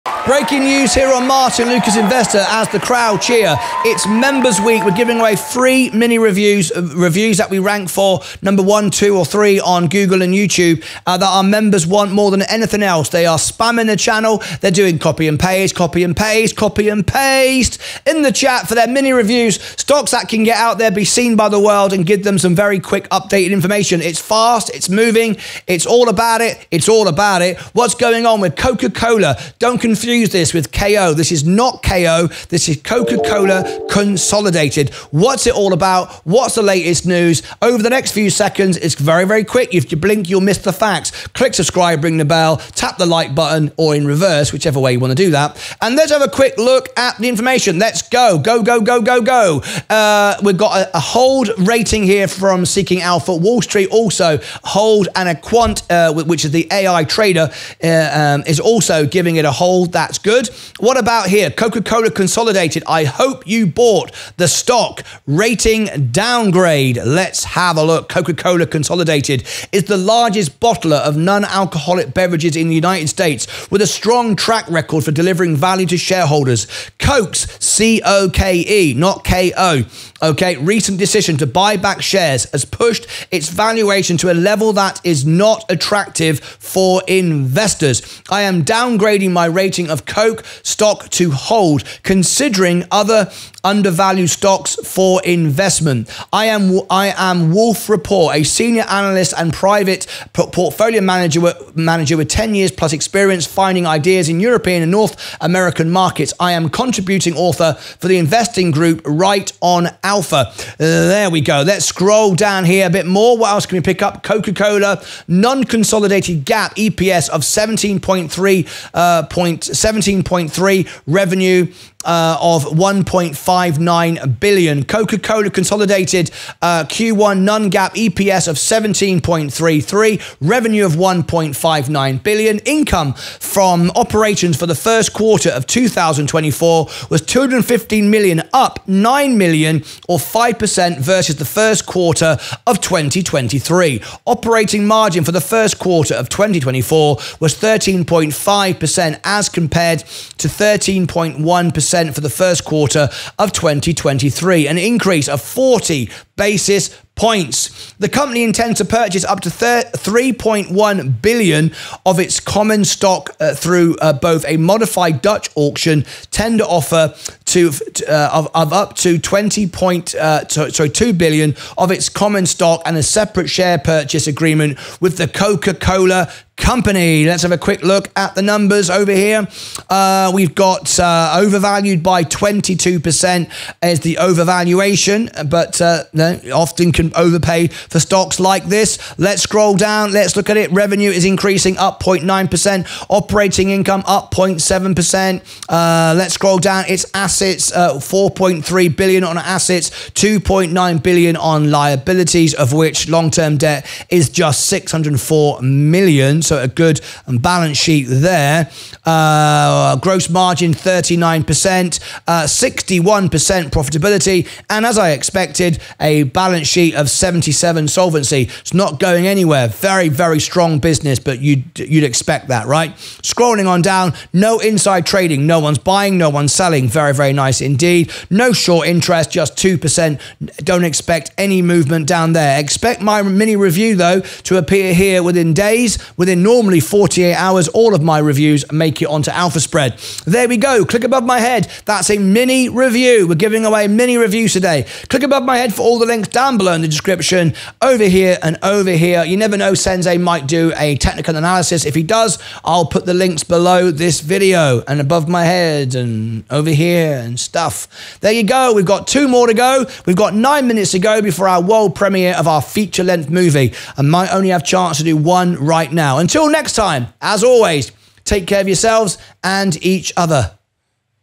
COKE Stock - Coca-Cola Consolidated MINI STOCK ANALYSIS - Martyn Lucas Investor
COKE Stock - Coca-Cola Consolidated MINI STOCK ANALYSIS

Coca-Cola Consolidated, Inc. engages in the production, marketing, and distribution of nonalcoholic beverages. Its products include sparkling and still beverages as well as energy products and noncarbonated beverages such as bottled water, tea, ready to drink coffee, enhanced water, juices, and sports drinks. It operates through the Nonalcoholic Beverages, and All Other segments. The company was founded by J. B. Harrison in 1902 and is headquartered in Charlotte, NC. The listed name for COKE is Coca-Cola Consolidated, Inc. Common Stock

FREE NEWSLETTER

Enter email. If you don't see an auto response search in your junk folder

Sensei Crypto
Crypto Challenge.